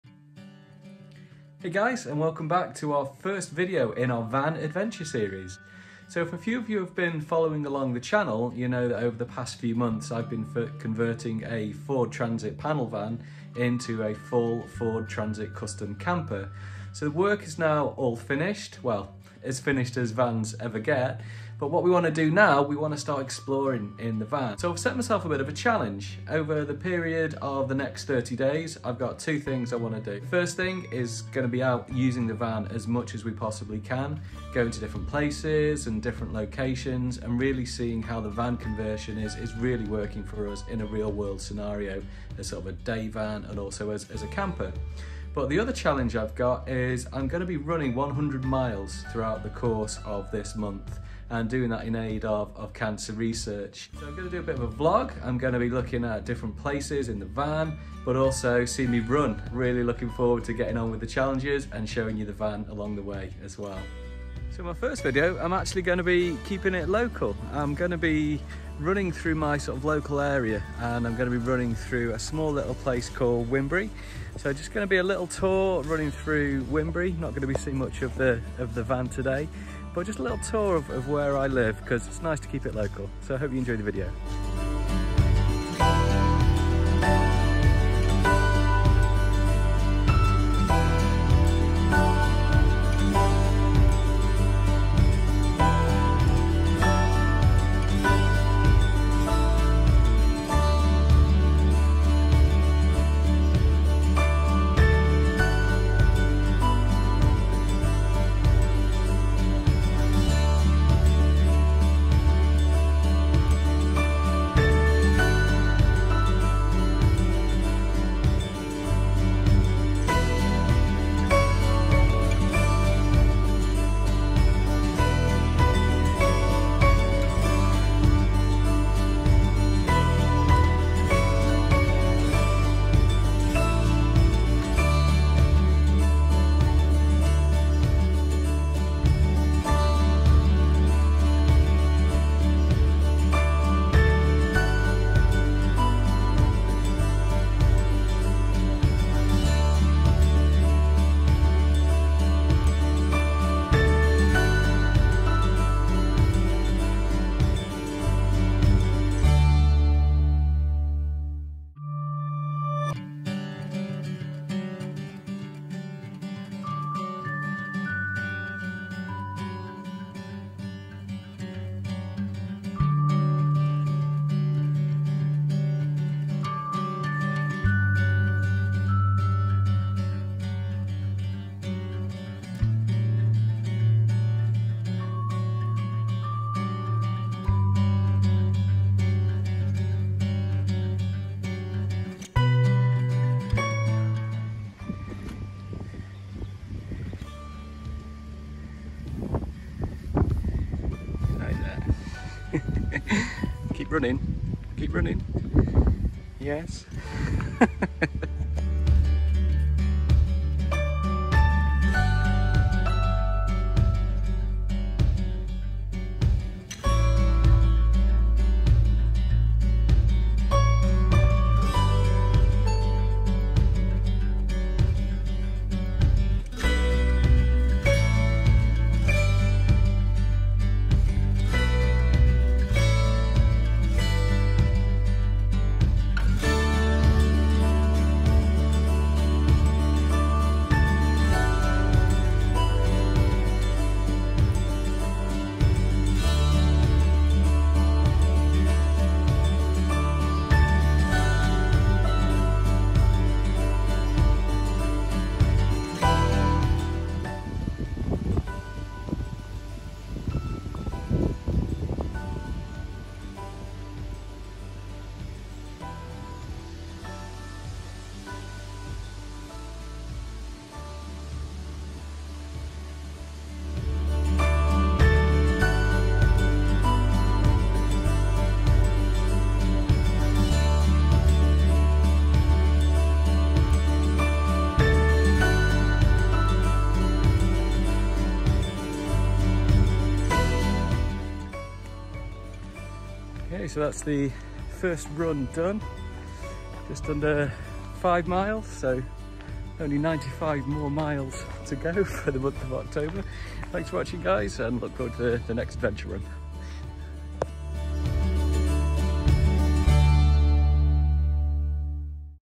#1 | Start of Transit Custom Camper Adventures, Starting 100 Miles Run & Wybunbury Tour
Starting the van adventure series using our Transit Custom Camper as a base for the running 100 miles in October challenge. First is a tour of my local area Wybunbury Moss. Will also be looking at the van and how is stands up to daily use.